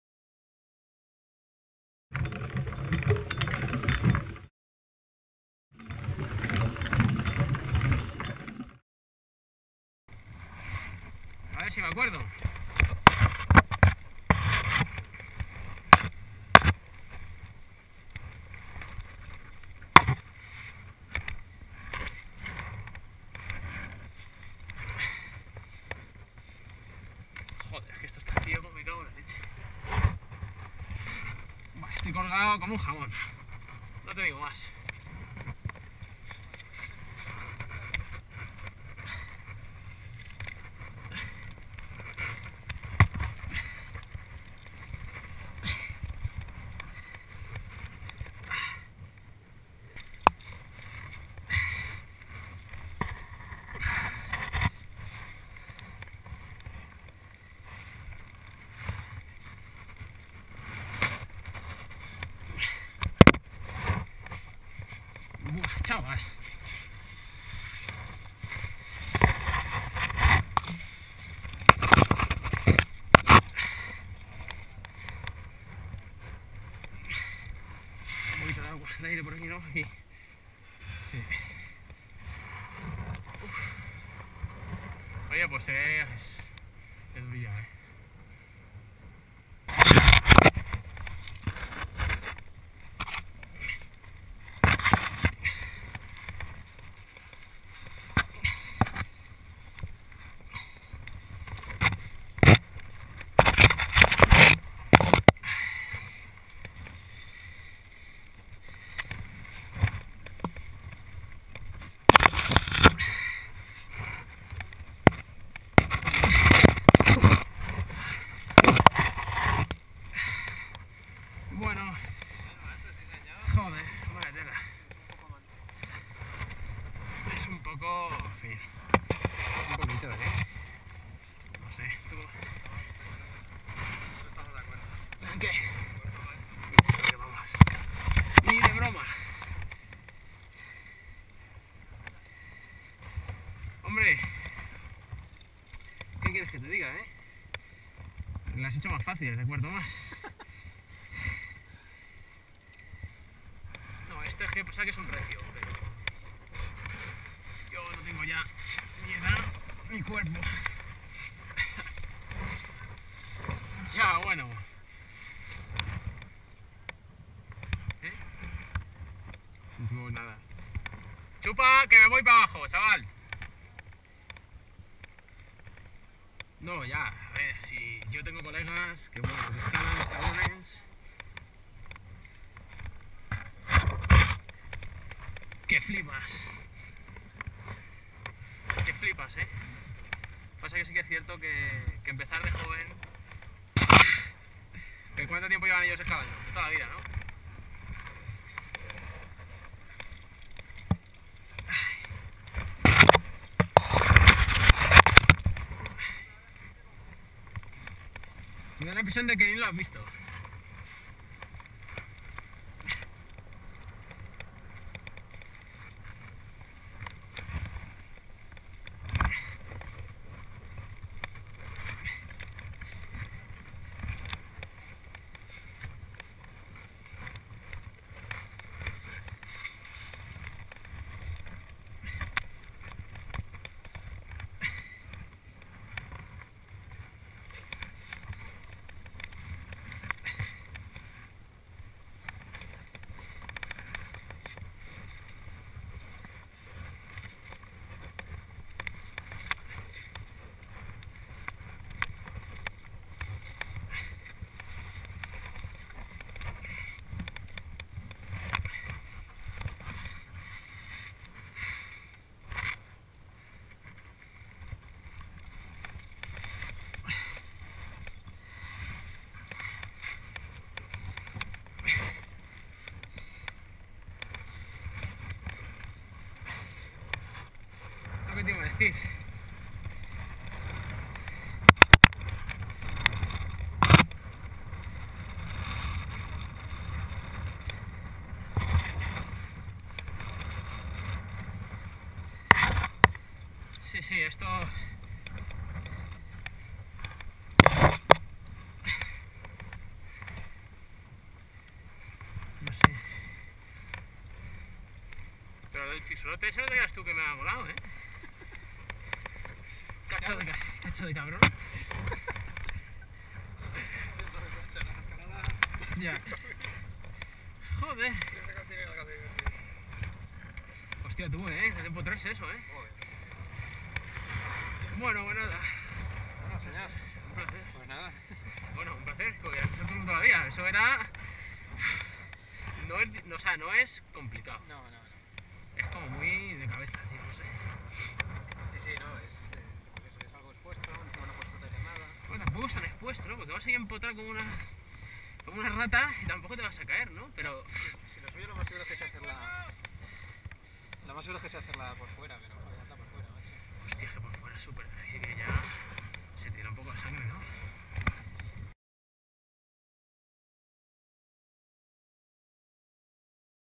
Via Chelo IV+ Cinco Cestos
Via de IV+ a la derecha de cinco cestos, que recorre la chorrera por la que cae el agua de la pared. El primer tramo no será mas de III, pero luego se va poniendo vertical y hay que apretar. Cacharros grandes para asegurar (via limpia excepto reuniones). Dos rápeles te ponen en el suelo.

Primera via hecha con Vlady Bústof (Historias de Montaña) después de casi 6 (o más) años hablando de quedar para escalar :-)